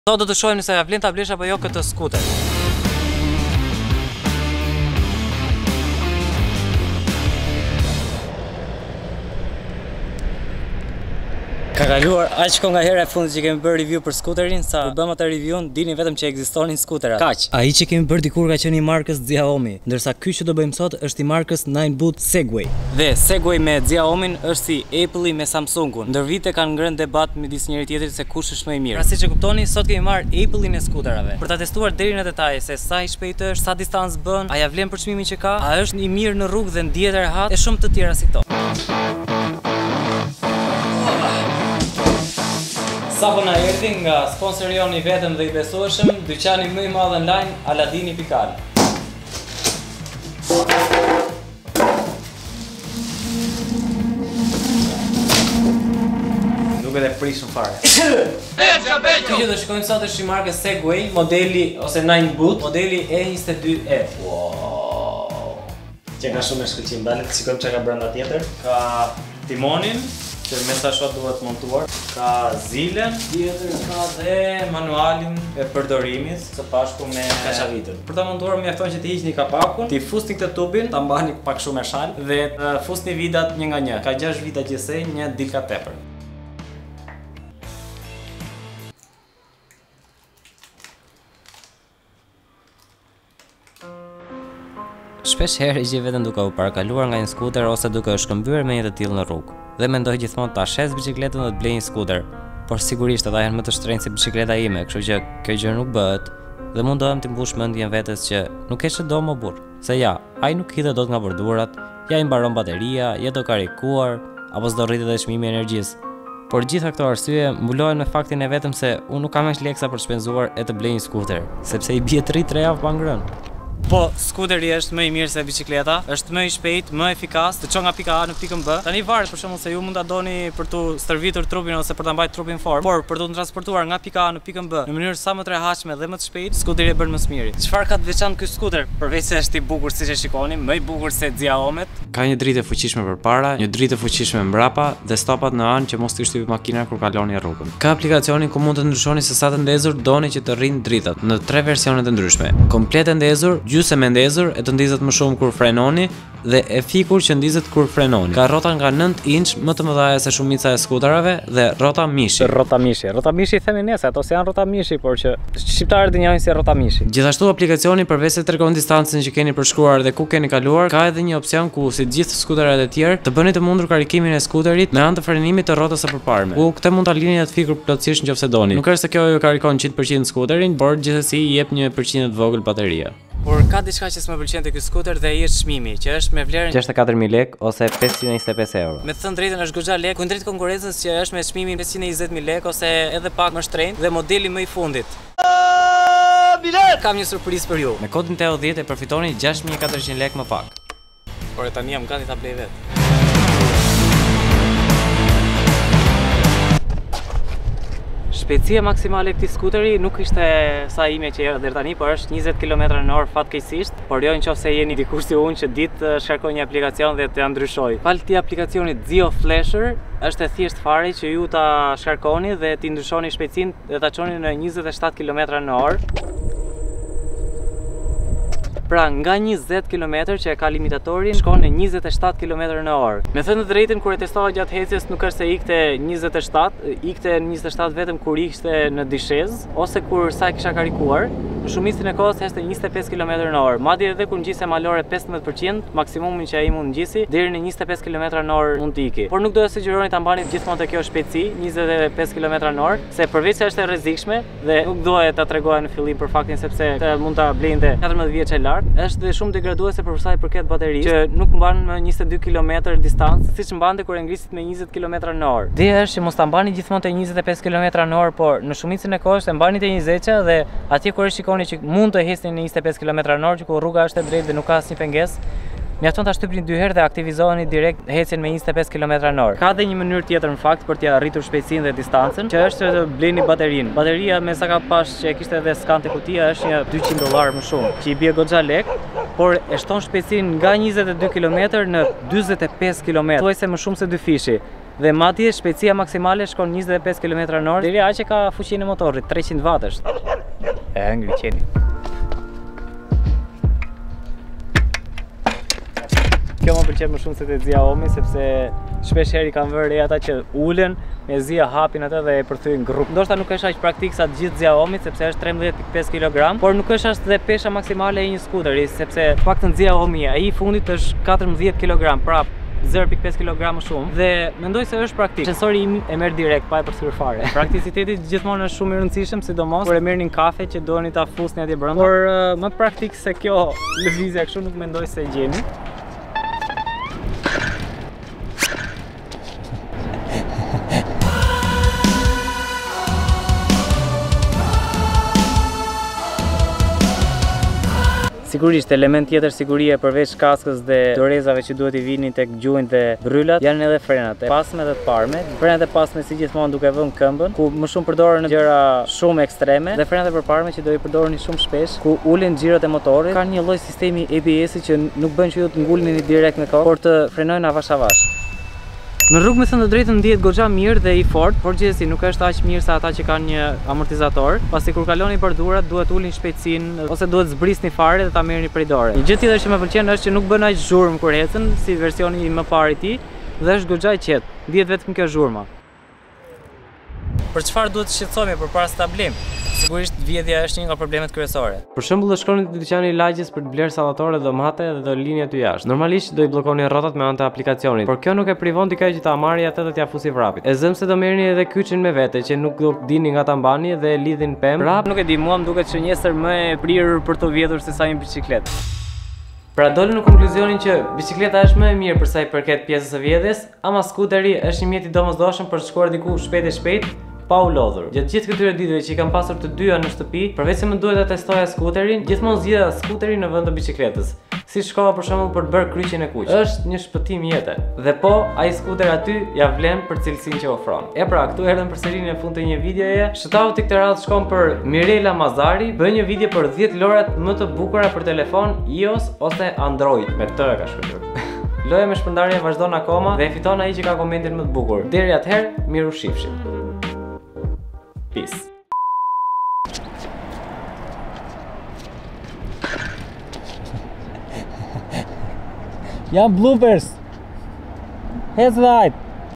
$610 Skuter Elektrik !
A ja vlen ta blesh kete skuter elektrik Segway Ninebot E22E $610?

Kodi per 10% ULJE : TEO10

Nese ju ka pelqyer video dhe keni mundesine per te ndihmuar ne realizimin e akoma me shume videove me te bukura,mund te kontriboni ne linkun e meposhtem  :

6:27 Cili eshte problemi?